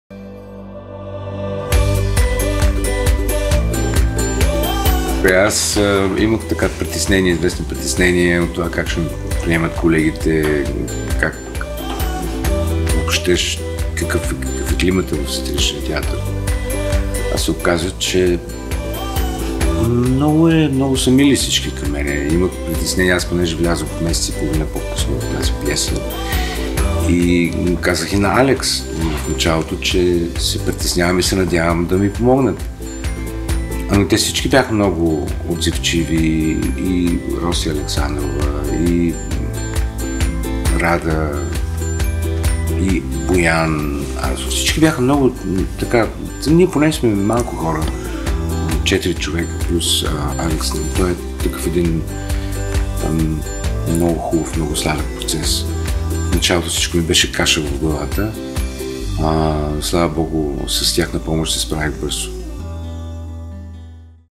Димитър Баненкин - 1
Вихрушка
от Йерун ван ден Берх
Превод Нина Сиракова

Постановка Александра Петрова
Сценография Десислава Банкова
Участват Димитър Баненкин, Росица Александрова, Рада Кайрякова, Боян Арсов